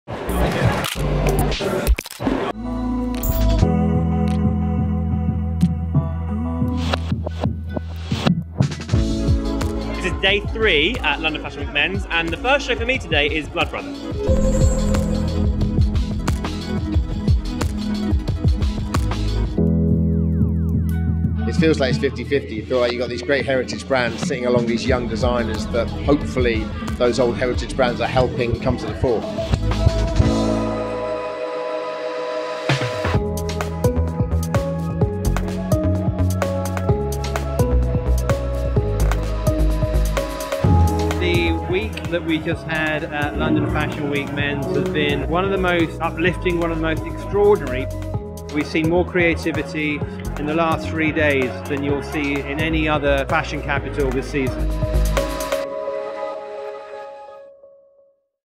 LFWM January 2018 | Day 3 Highlights
See highlights from the final day of London Fashion Week Men’s including Blood Brother, Per Götesson, TIMOTHY HAN/EDITION, Belstaff, Bobby Abley ; plus British Fashion Council NEWGEN recipients Nicholas Daley and A-COLD-WALL*.